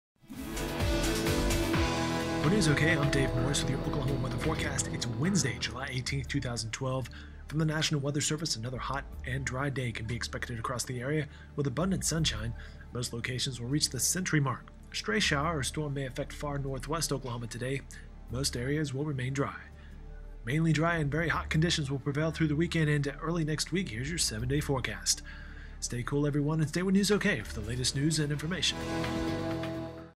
Oklahoma Weather Forecast: Wednesday, July 18, 2012 (2012-07-18)
Another hot and dry day can be expected across the area today with abundant sunshine. Most locations will reach the century mark.